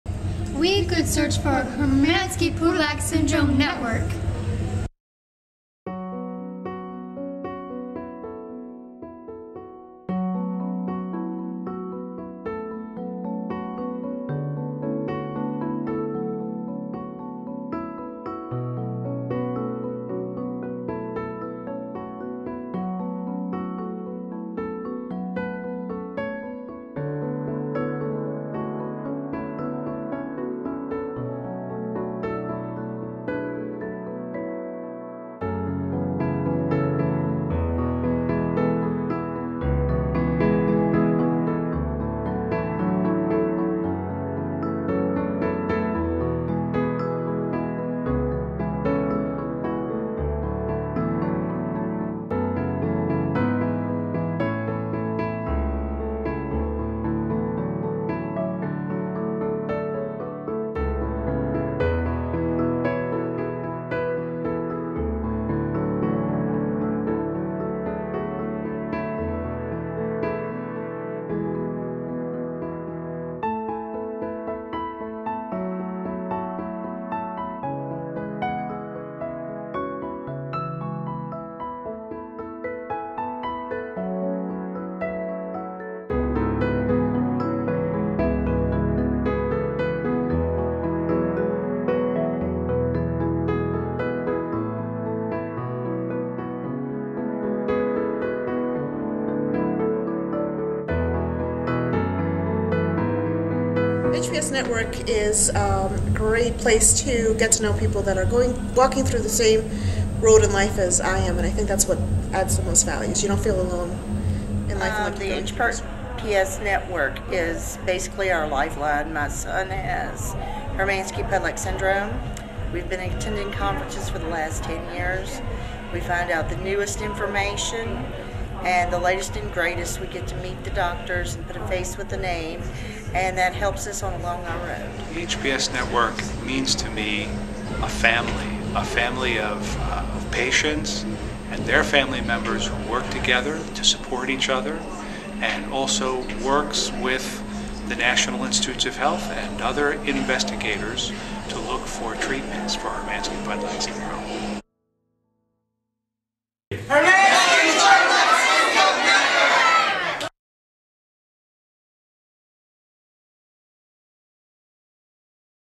I GoodSearch for The Hermansky-Pudlak Syndrome Network
Good News!!
The "I GoodSearch for the Hermansky-Pudlak Syndrome" video won the GoodSearch video of the week on Friday, April 25, 2008.  The HPS Network will receive 100 dollars from GoodSearch, and was on the HOMEPAGE of GoodSearch all day Friday!!!

THANK-YOU, GOODSEARCH!!!

HPS is a rare, genetic, metabolic disorder which causes albinism, visual impairment, platelet dysfunction with prolonged bleeding, and progressive symptoms including pulmonary fibrosis, inflammatory bowel disease, and kidney disease.